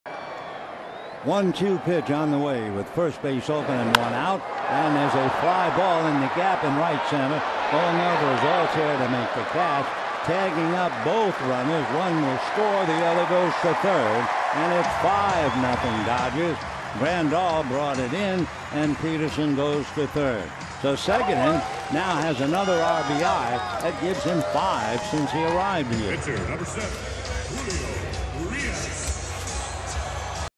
PHI@LAD: Segedin plates Grandal with a sacrifice fly
Rob Segedin lifts a sacrifice fly to right field, scoring Yasmani Grandal for the Dodgers' fifth run of the 1st inning